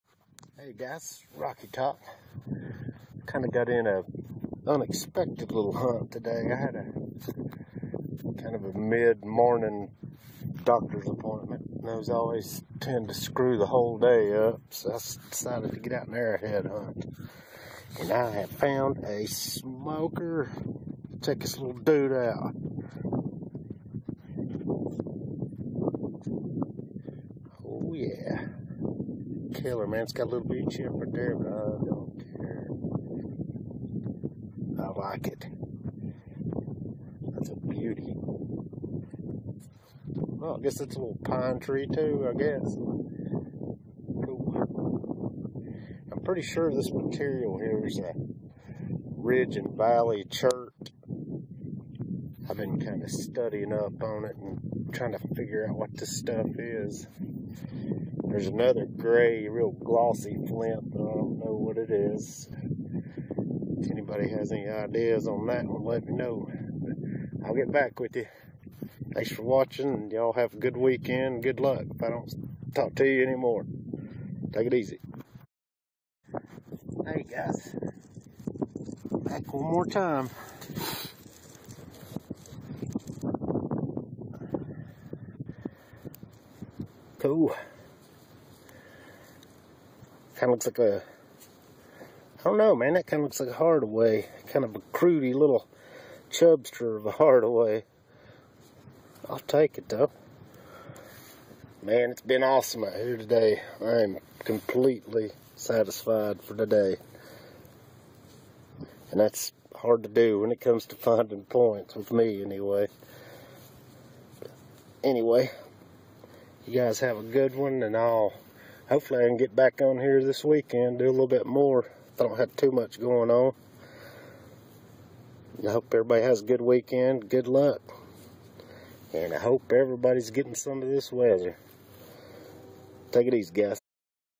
Arrowhead hunting on Friday!
Got out and found some cool ones...got a few more to show,but i will put them on this weekends video..take it easy! Good luck!!